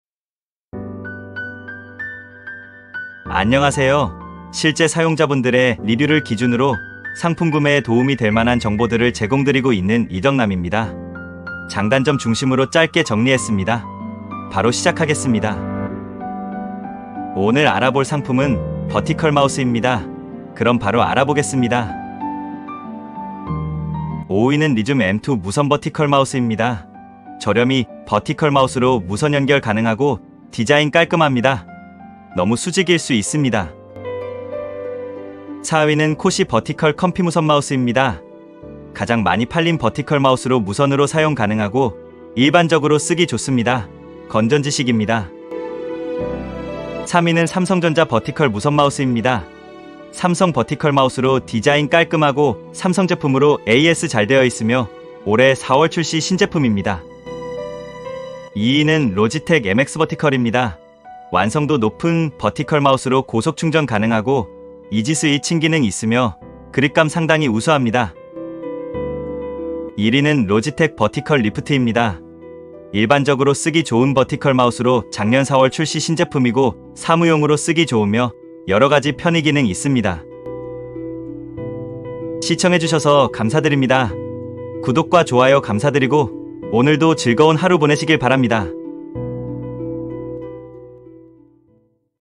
버티컬마우스 추천 2023 3분기 최신 Top 5 (리뷰 간단 정리)
0:25
5위
리줌 M2 무선버티컬마우스 (1만 1천원)

0:37
4위
코시 버티컬 컴피 무선마우스 (1만 8천원)

0:48
3위
삼성전자 버티컬 무선마우스 (3만 6천원)

0:59
2위
로지텍 MX VERTICAL (12만 4천원)

1:11
1위
로지텍 버티컬 LIFT (7만 8천원)